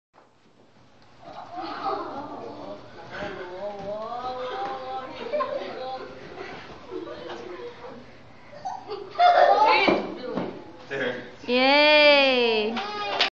Flare, cricket, two flares, cricket to hollow back combo for the 3rd and 4th graders at break!!!
A demonstration to encourage the students to be fit!!!